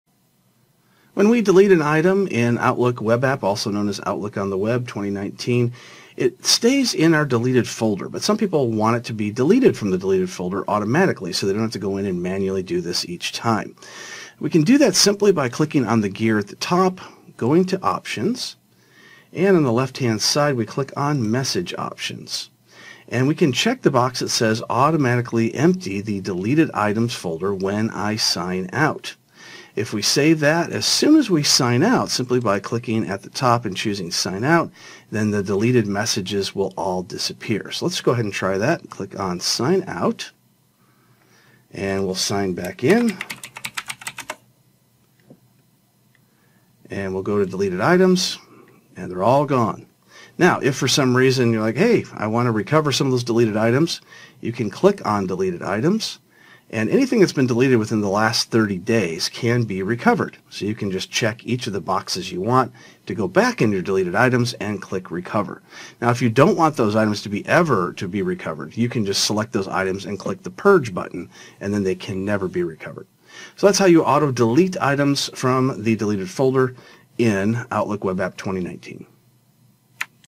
How to auto delete email from deleted items in Outlook Web App (OWA) 2019
Professor Robert McMillen shows you how to auto delete email from deleted items in Outlook Web App 2019